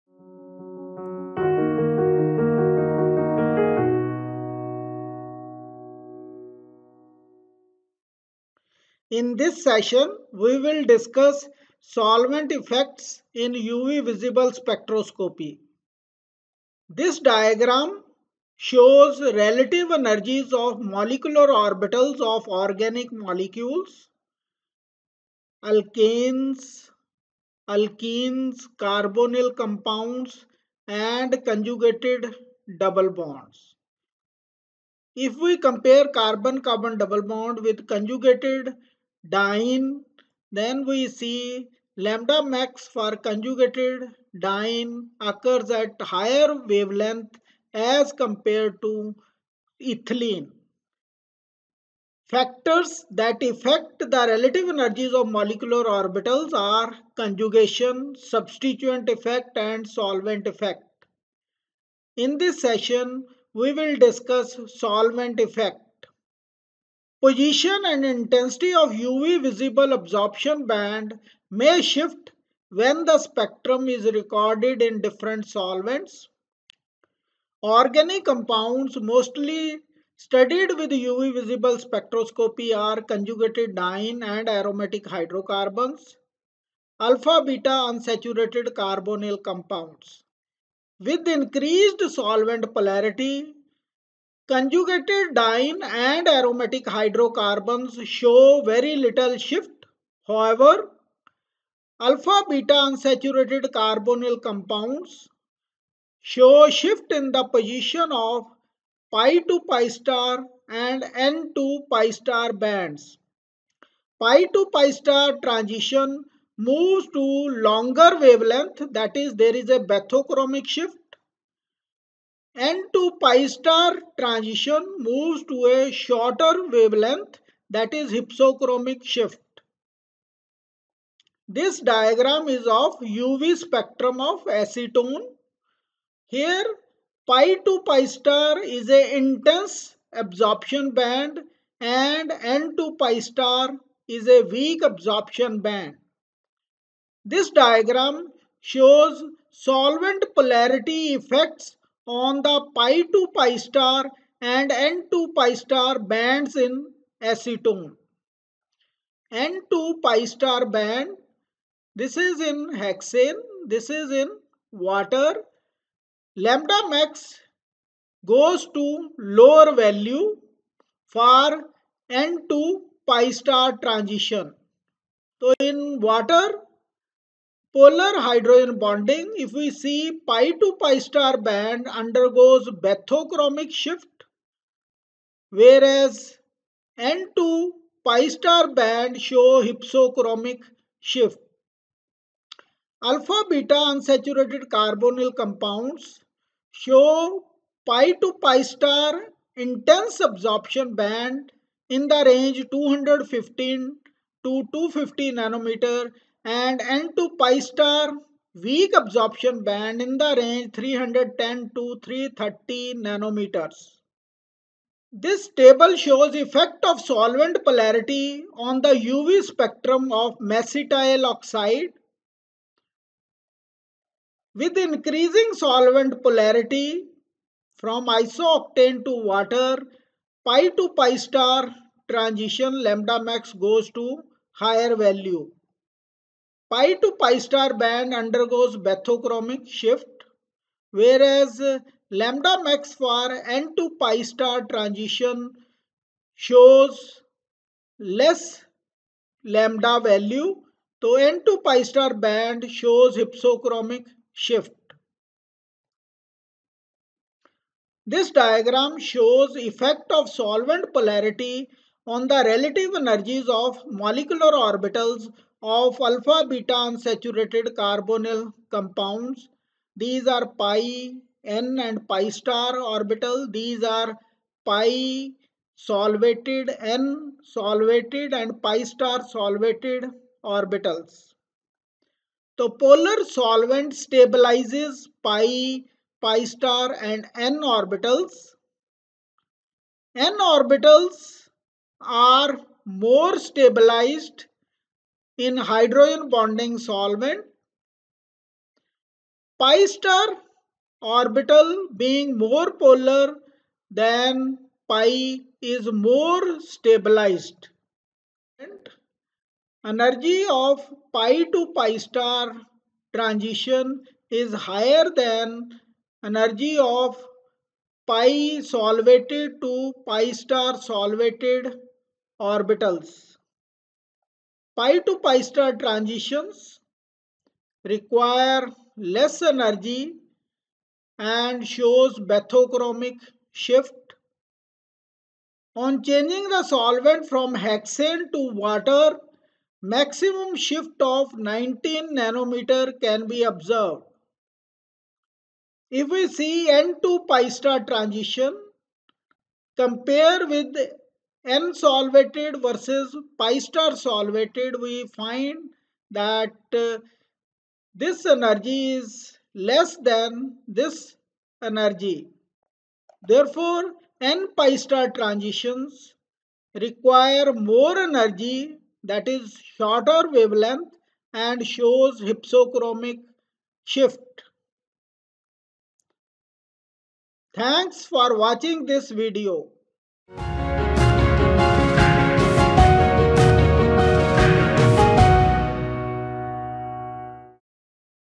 Solvent Effects in UV Visible Spectroscopy
This video is for UG and PG science students and discusses about effects of solvents in UV-Visible Spectroscopy